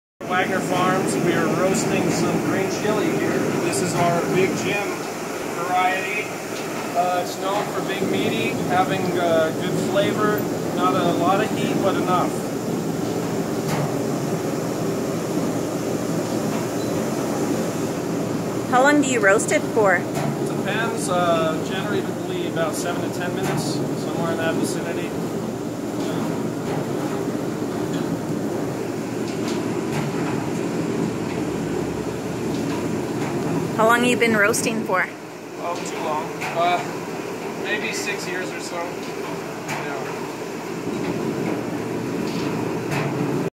Green Chile roasting at Wagner farm in Corrales part 2
Part 2 Big Him Green Chile roasting with Roman (best Chile roaster of all time)